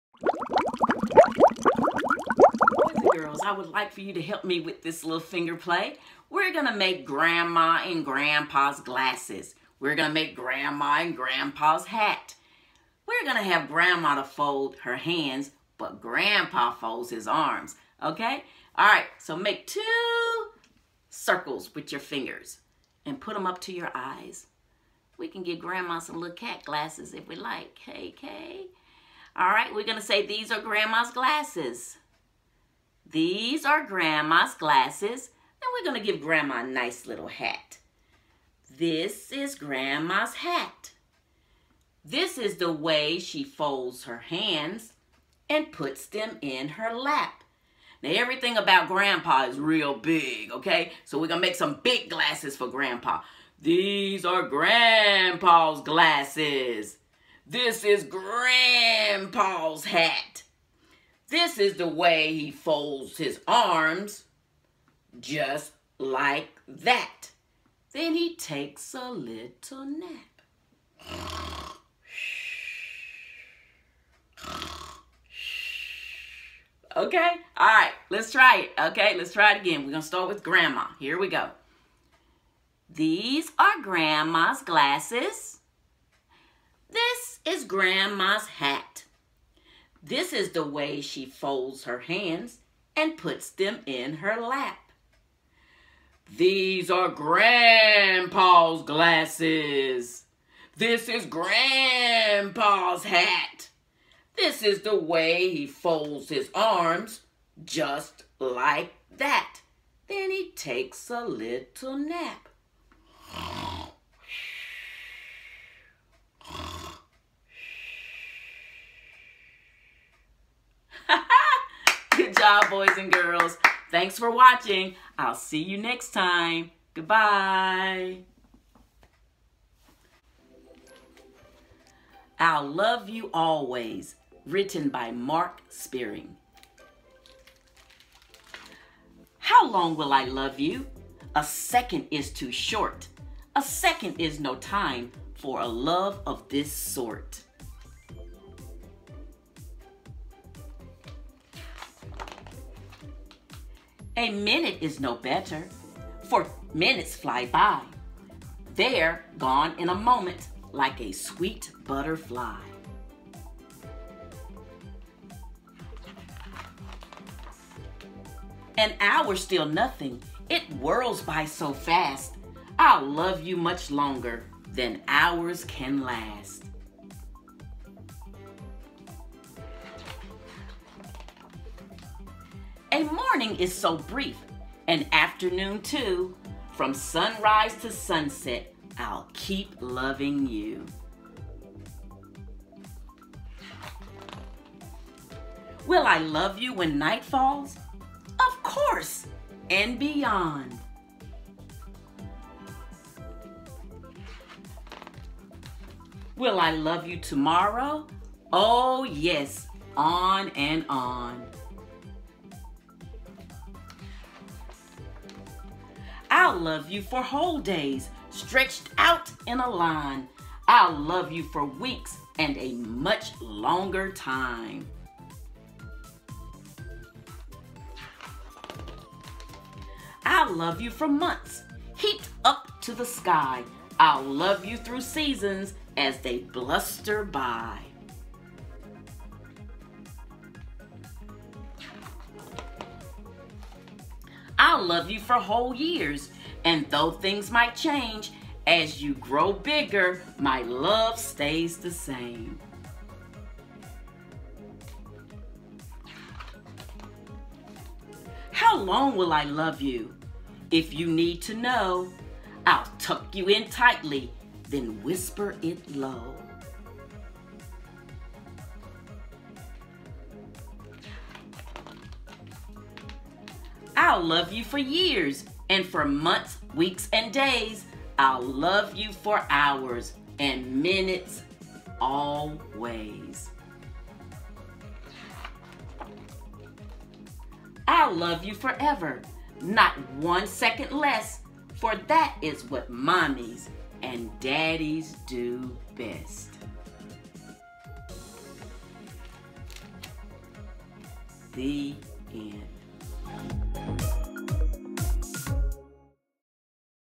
Mini Story Time with Ms. Delisa at Lee County Libraries 4/30/20
Mini Story Time features the finger play Grandma & Grandpa and the story “I'll Love you Always” by Mark Sperring